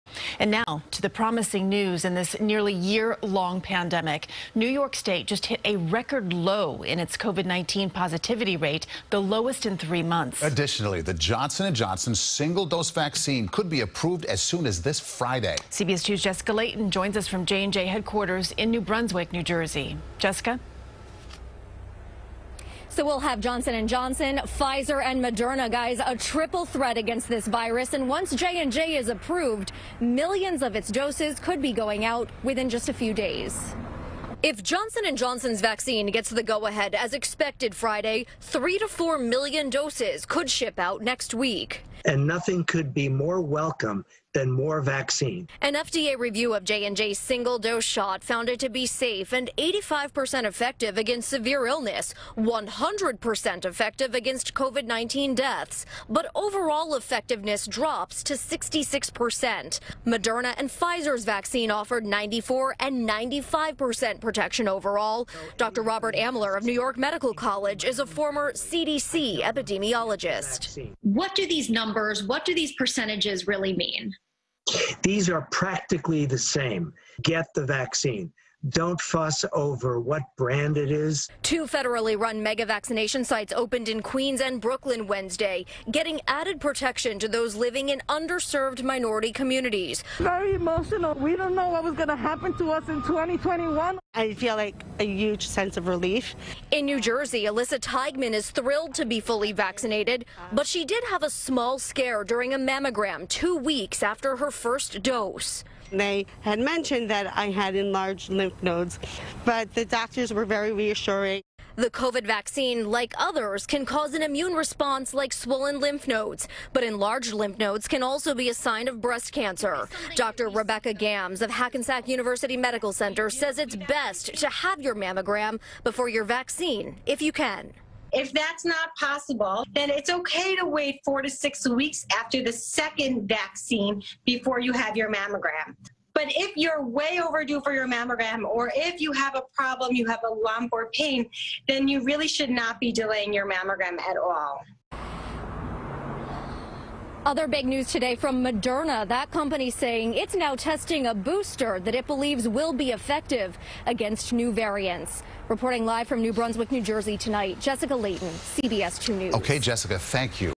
New York State's COVID Positivity Rate Drops Again With Johnson & Johnson Vaccine Approval On Horizo
New York State's COVID-19 positivity rate is now its lowest in three months. This good news comes as the Johnson & Johnson single-dose vaccine could be approved as soon as this Friday; CBS2's Jessica Layton reports.